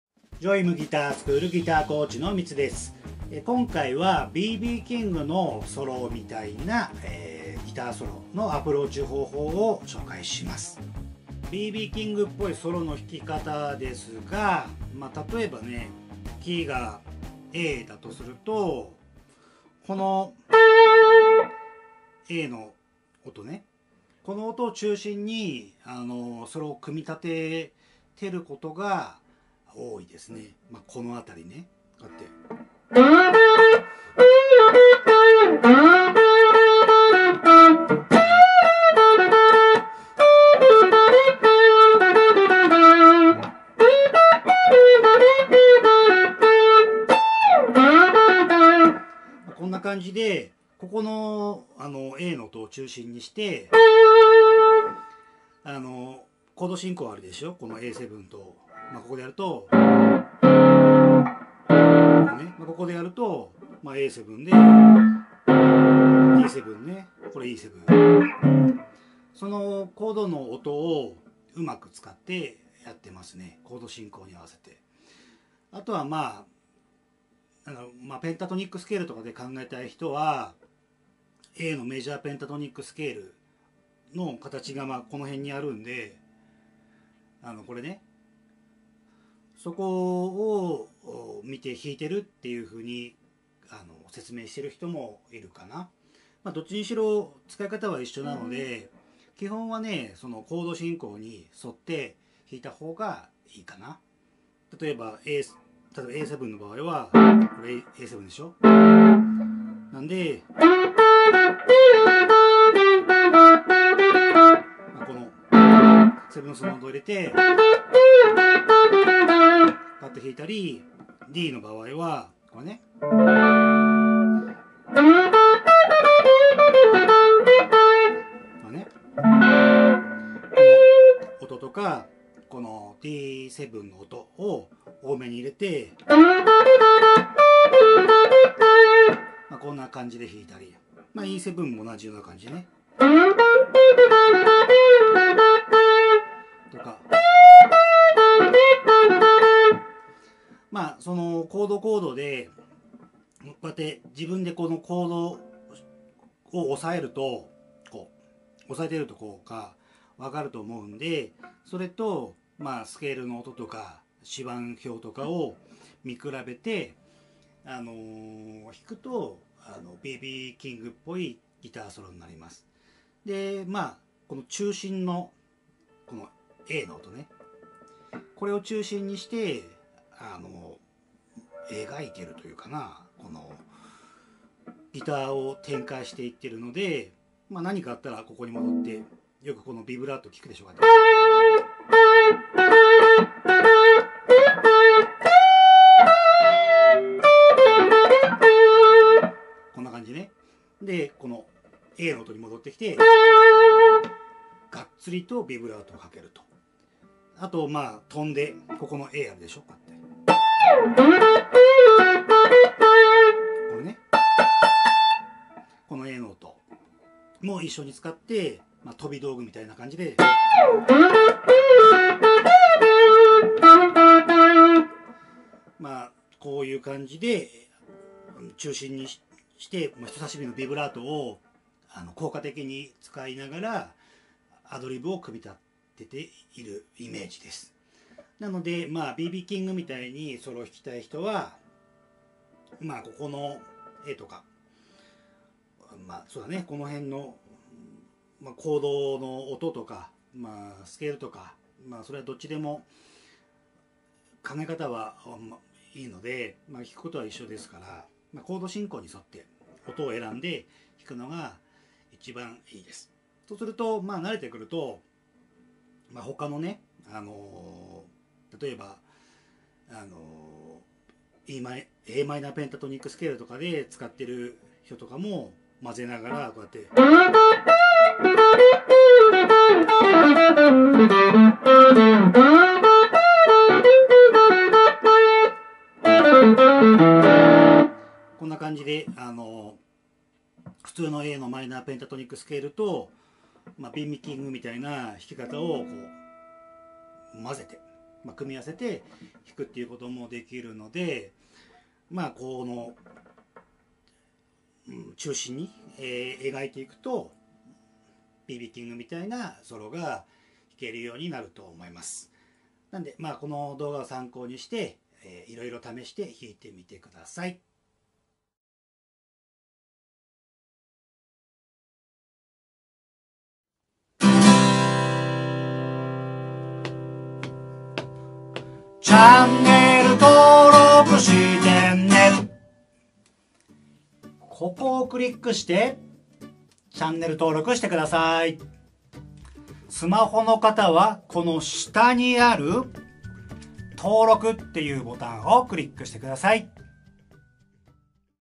ブルースギター初心者レッスン【B.B.Kingのソロが簡単に弾ける方法を解説！】
【BLUESギターオンラインレッスン】は↓こちら

【簡単ステップ式】だから、ブルースギターが楽しく上達♪
【会員オンラインレッスン】10日間無料体験は↓こちら

ブルースの王様「B.B.King」のギターソロの弾き方を解説しているレッスン動画です。

B.B.Kingのソロはかっこいいですよね～。

特徴は、
「人差し指のビブラート！」
「コードトーンをうまく使っている！」
「メジャーペンタを使っている！」
などなど。

いろいろありますが、今回はB.B.Kingのソロの完コピというよりは、
「どうやって弾いているのか？」
というアプローチ方法（B.B.Kingスタイルのソロの弾き方）を簡単に説明します。

動画の画面上にスケール表が表示されますので、
それも参考にするとギターソロの弾き方が分かりやすいですよ。

さあ、かっこいいソロを弾こう！